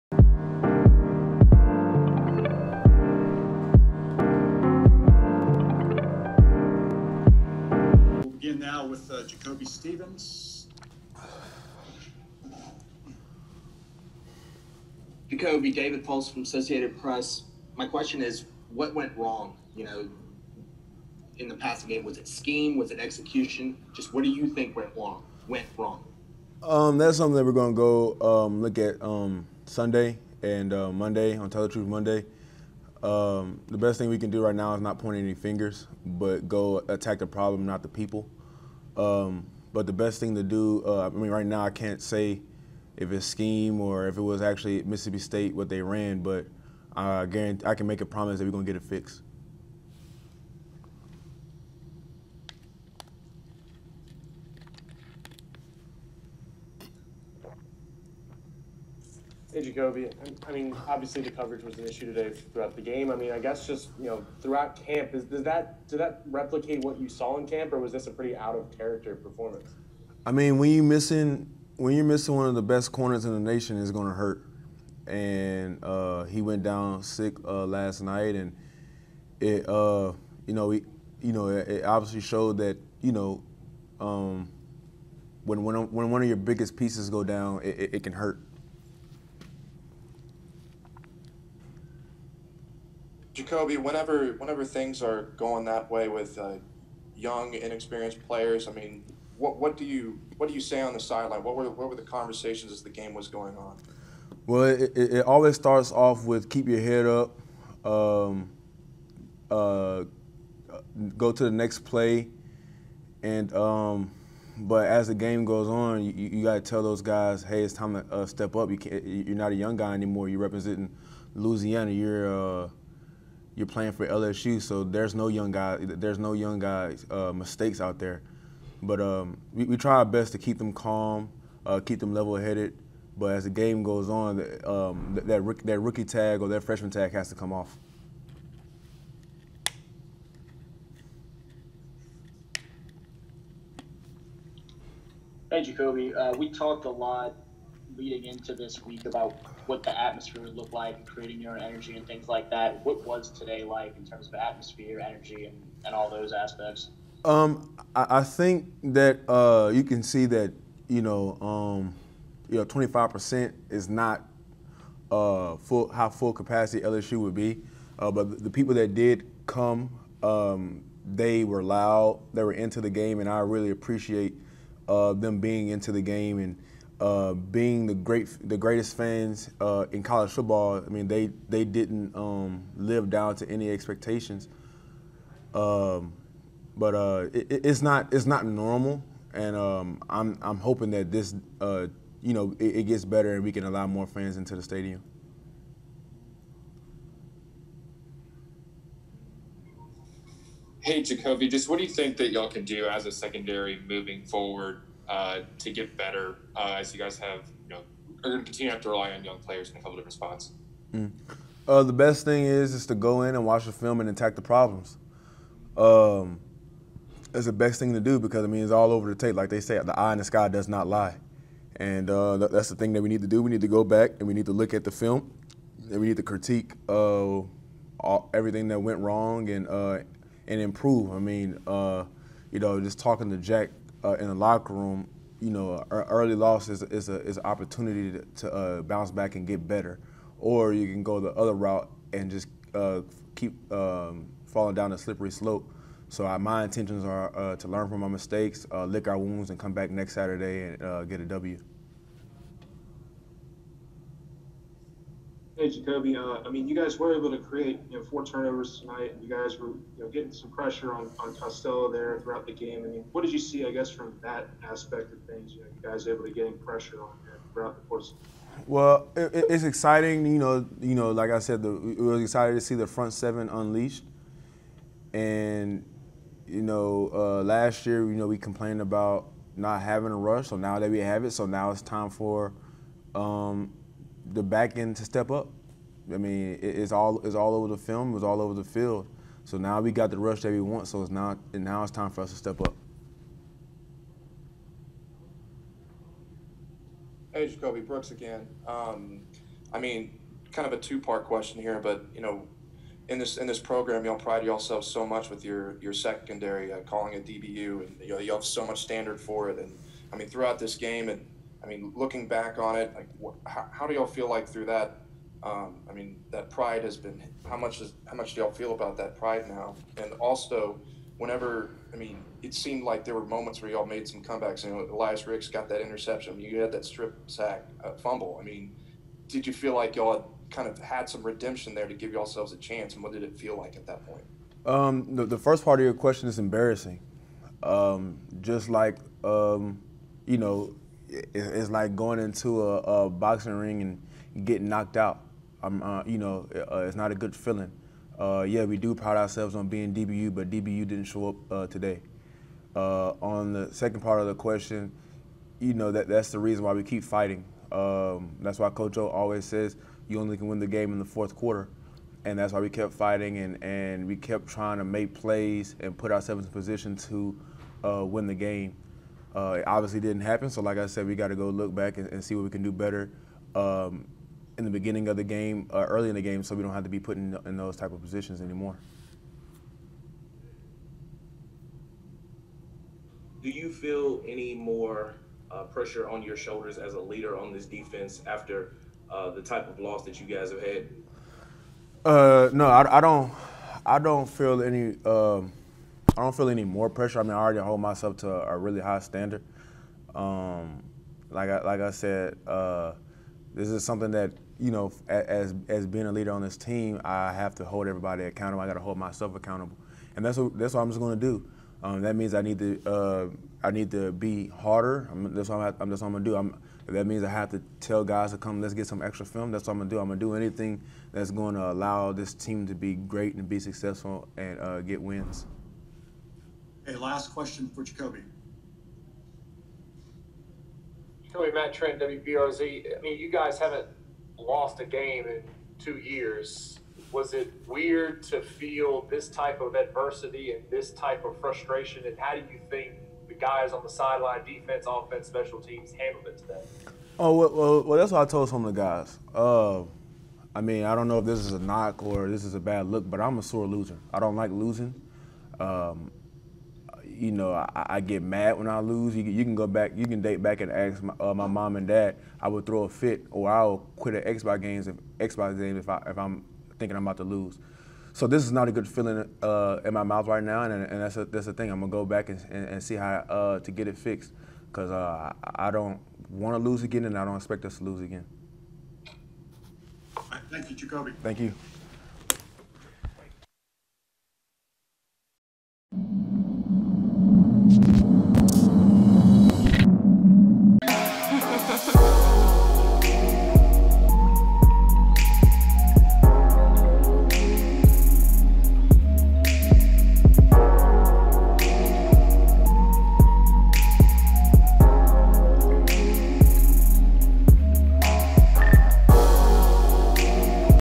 LSU's Jacoby Stevens Reacts to Tigers Surprising Loss to Mississippi State: "Gonna Get This Fixed"
Watch as LSU's Jacoby Stevens Reacts to Tigers Surprising Loss to Mississippi State: "Gonna Get This Fixed"